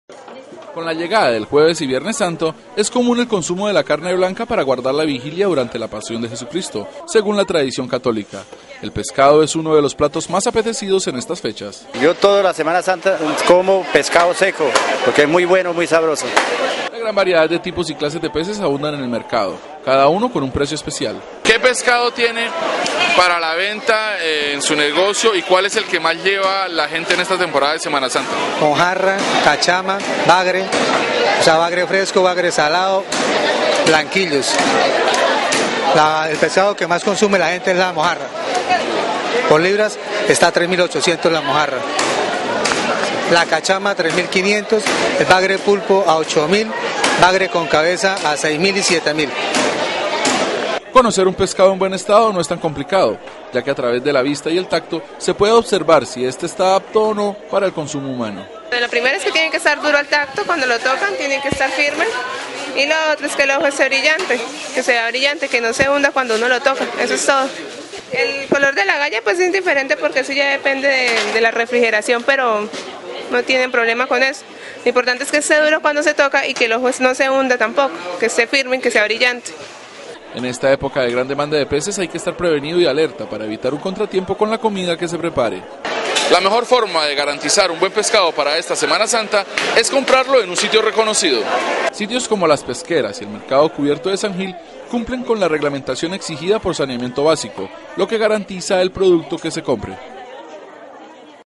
Tenga cuidado con el pescado que compre para semana santa
En semana santa es común el consumo de pescado durante los días que  se guarda la vigilia, sin embargo hay que tener cuidado con la compra para evitar intoxicaciones  y malos ratos.